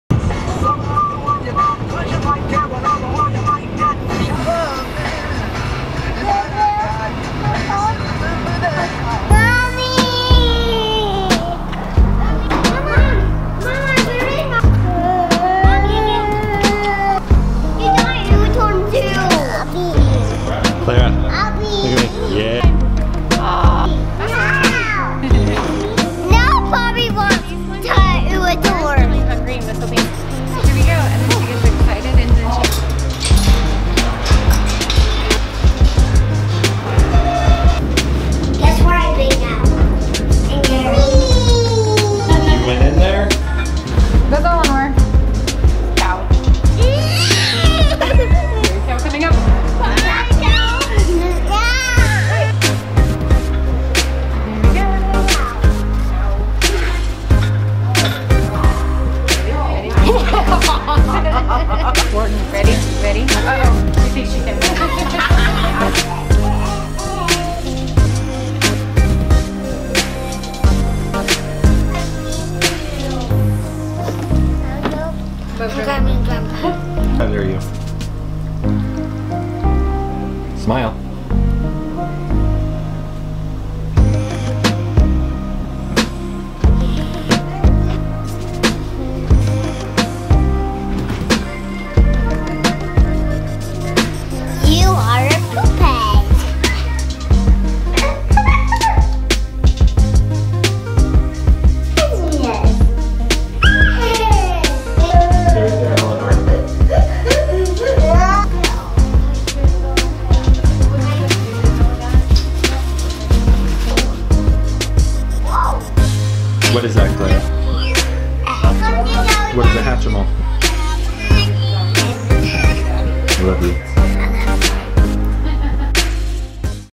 June 27, 2018
Clara gets a visit in the hospital from her sisters, Eleanor and Poppy, for dinner.